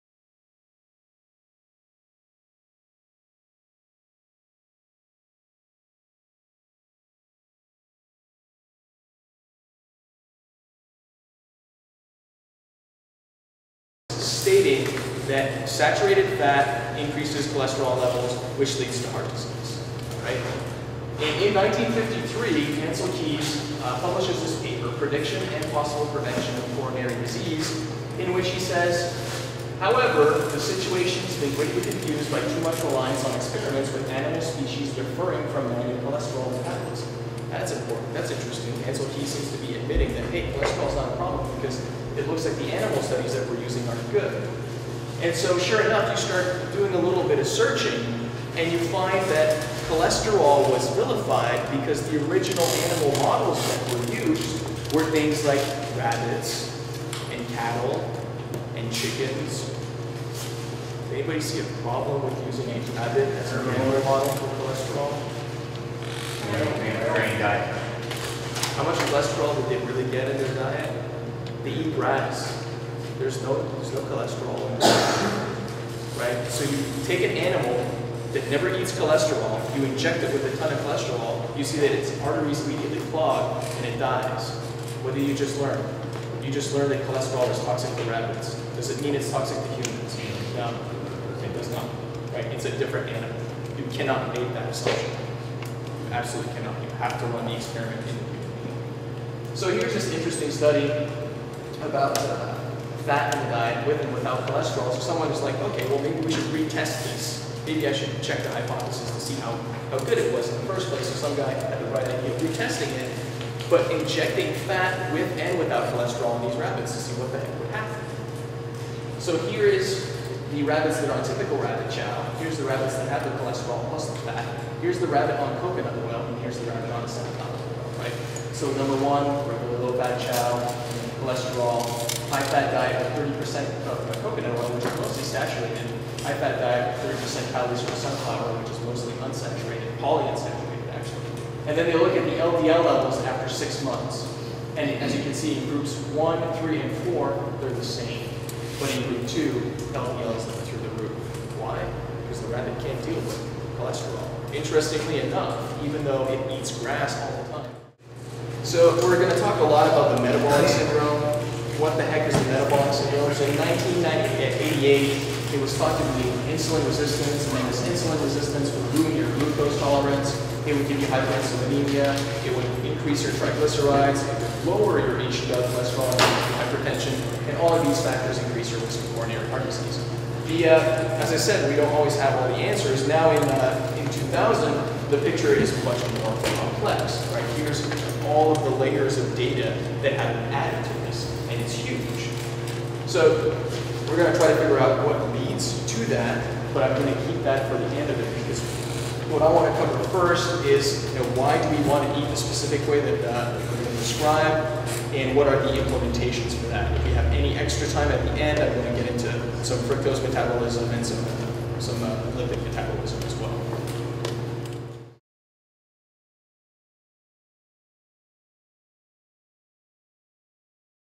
CrossFit Football - A Bit of Nutrition
Mathieu Lalonde gives a nutrition seminar at CrossFit Football's Orange County certification.  In this episode he discusses Fat Metabolism and the Metabolic Syndrome.